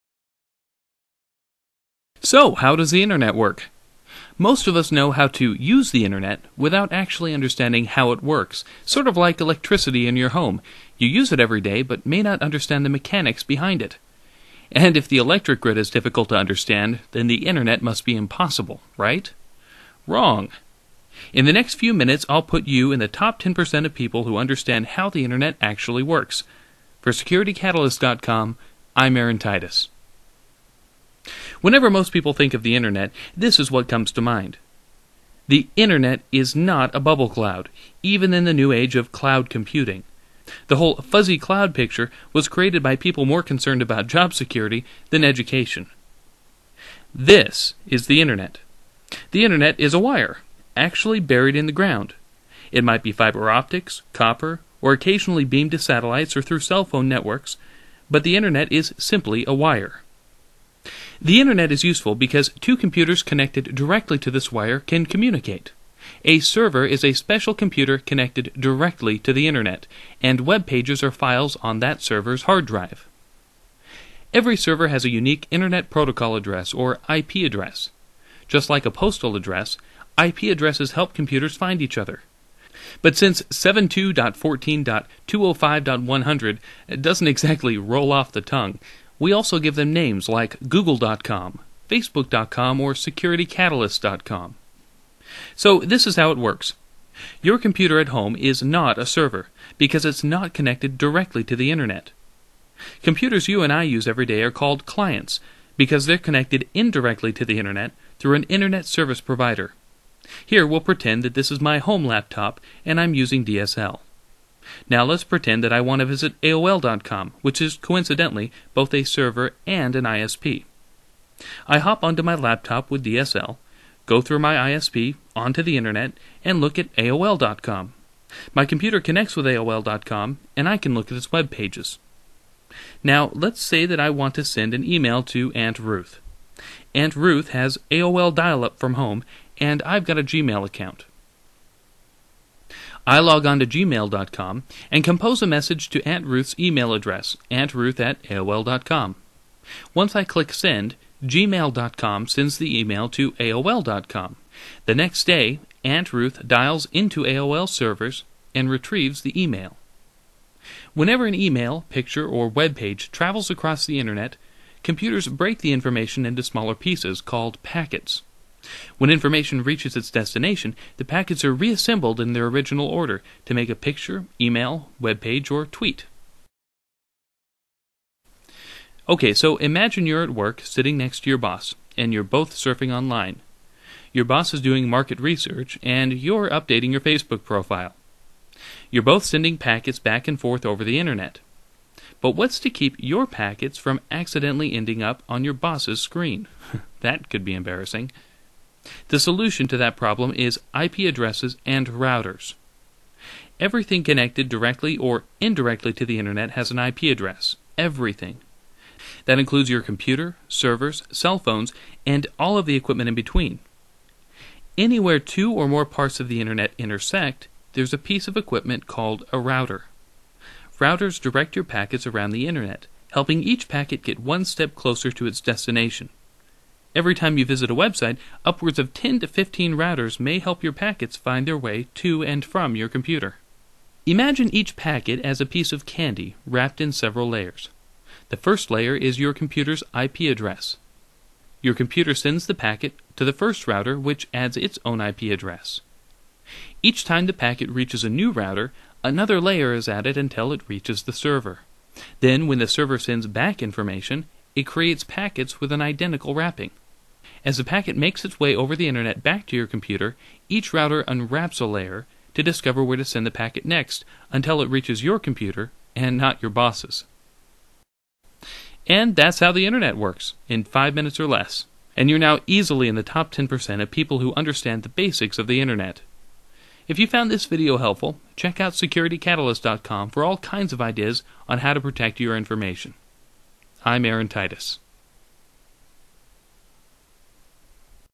How the Internet Works in 5 Minutes
The internet is not a fuzzy cloud. The internet is a wire, actually buried in the ground. Computers connected directly to the internet are called "Servers," while the computers you and I use are "clients," because they are not connected directly to the internet, but through an Internet Service Provider. Routers shuttle packets of information across the internet, and transmit e-mail, pictures, and web pages.